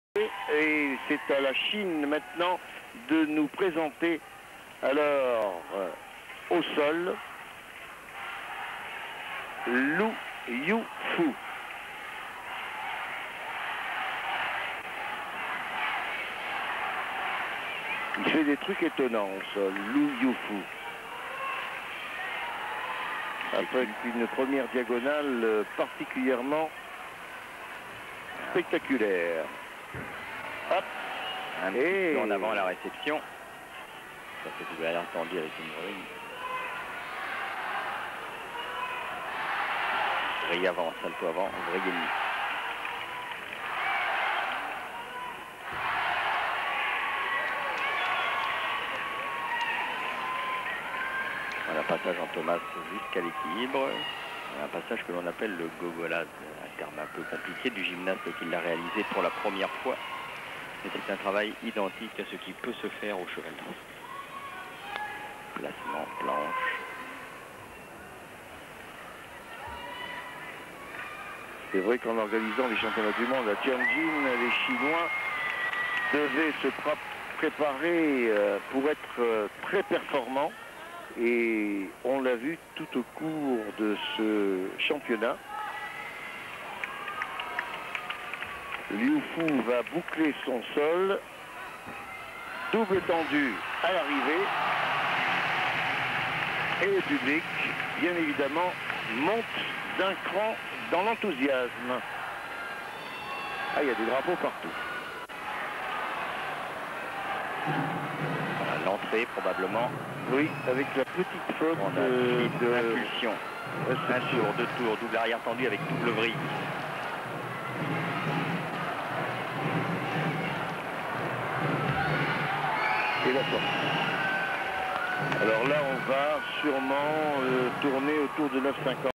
LU Yufu (CHN) floor - 1999 Tianjin worlds TF
LU Yufu (CHN)

'99 World championships in Tianjin - Team final

Team rank : 1st